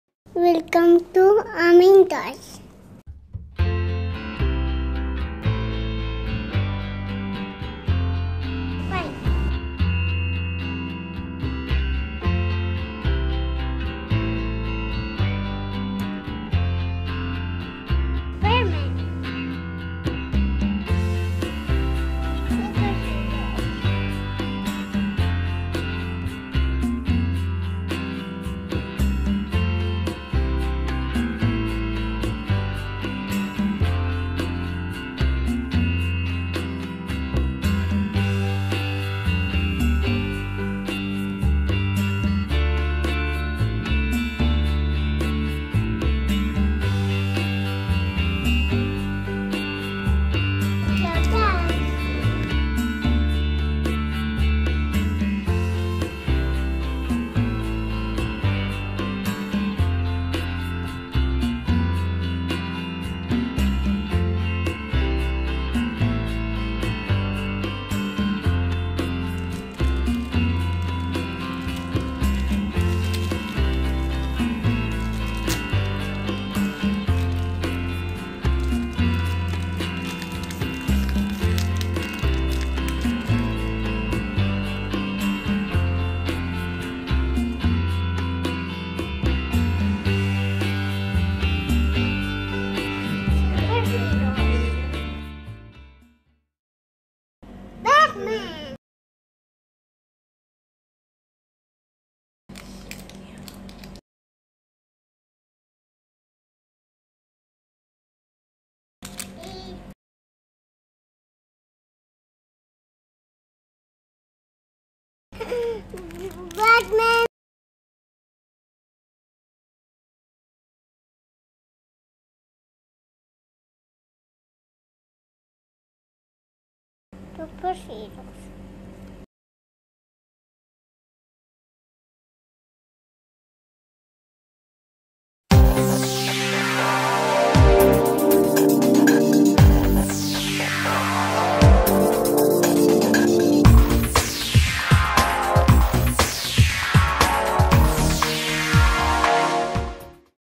Opening - Ameen Toys
Surprise Egg Opening by Ameen Toys (Recorded on 2 August 2018).

I, Ameen, youtuber started just before my second birthday . This is one of my toy review video. Your support is my greatest motivation.